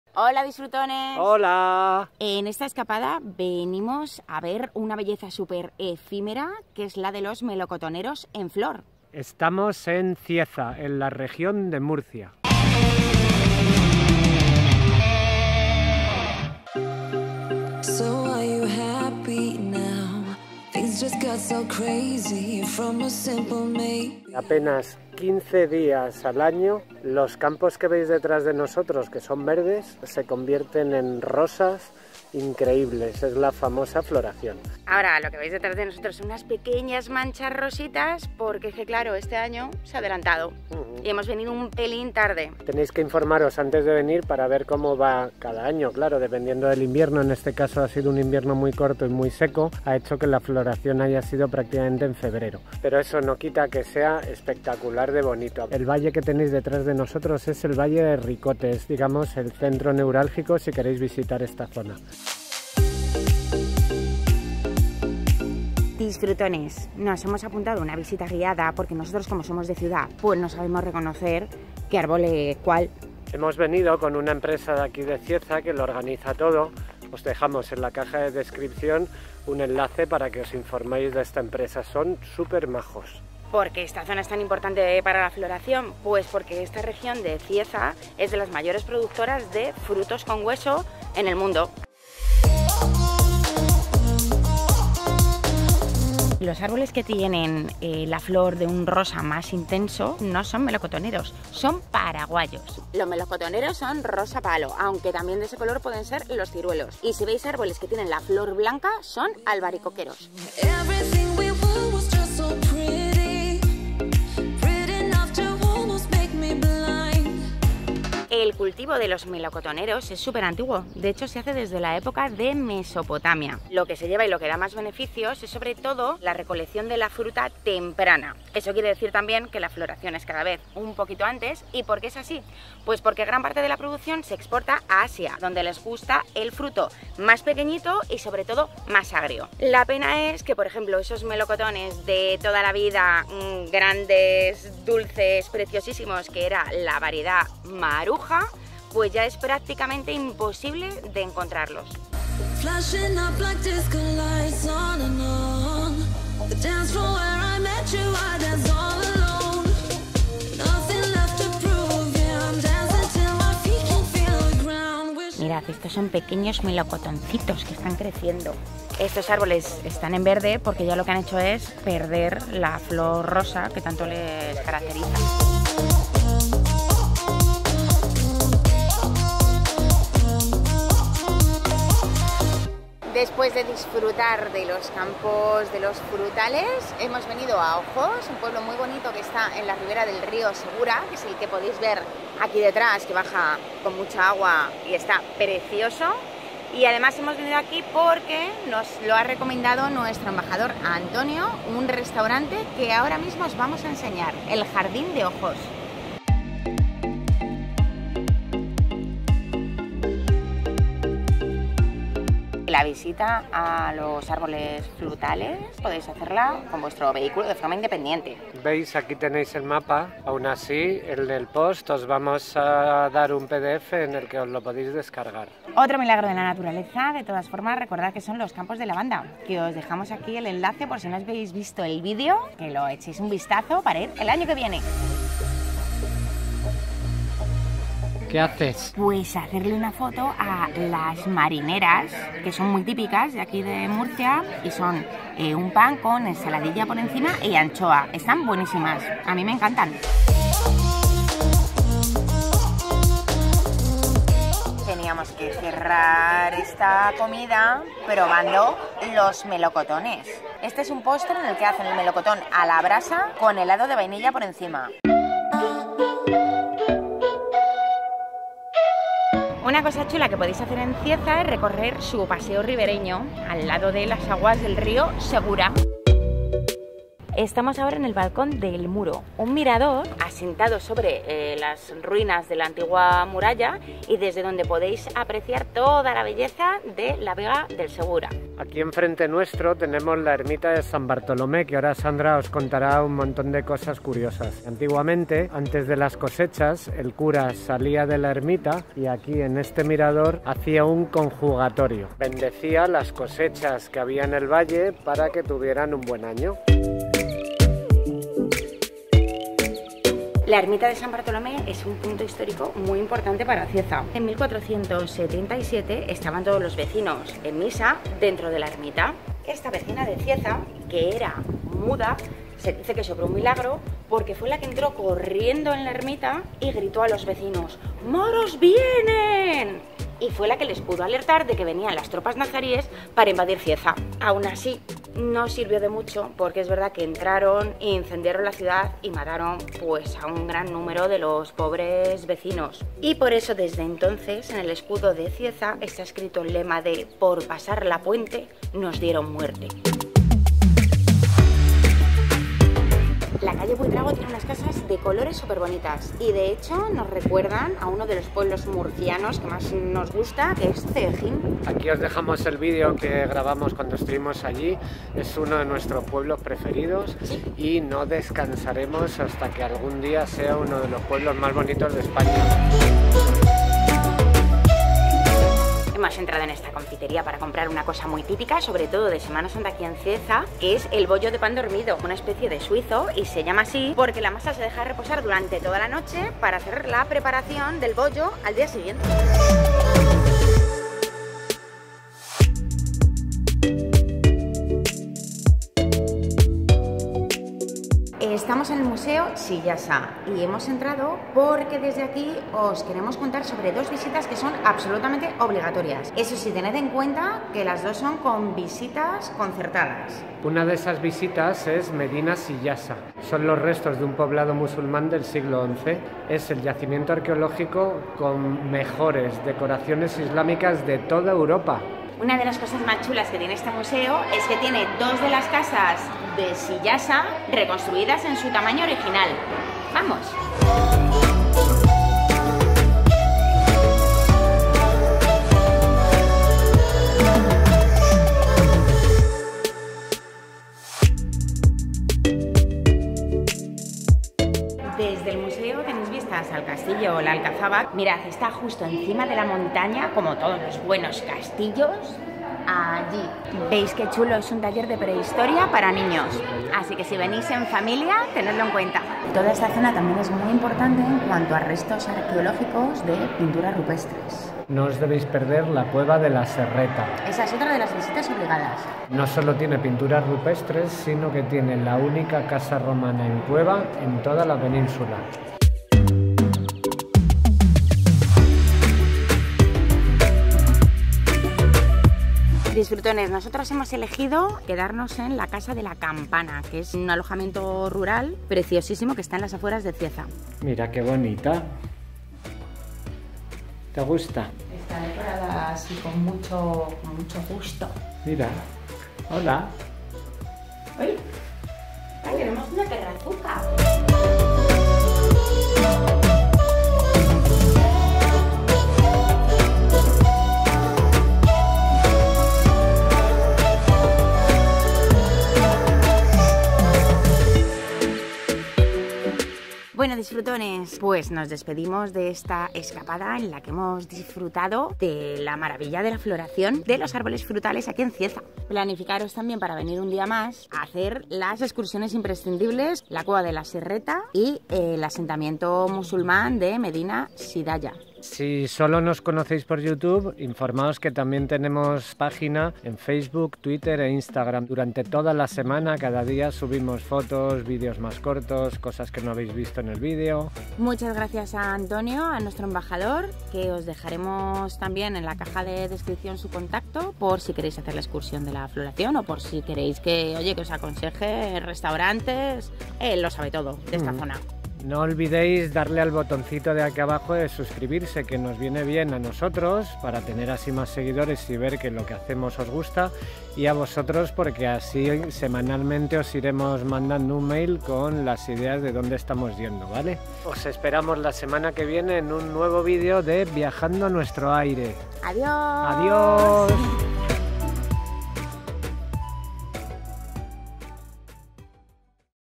¡La floración de Cieza!
¡Hola disfrutones!

En este video os mostraremos un ESPECTACULO UNICO, la FLORACION de los árboles frutales en CIEZA, Murcia.

No solo eso, sino que también os enseñaremos restaurantes, casas rurales y otras excursiones para disfrutar de esta maravillosa zona.

En el post que hemos subido a nuestra web os contamos todos los detalles: enlaces en los lugares de interés, restaurantes, las mejores fotos... Haz

Si os ha gustado el video, "regaladnos" un LIKE para saberlo y  COMPARTIDLO con vuestros amigos!

¡Hasta la próxima semana!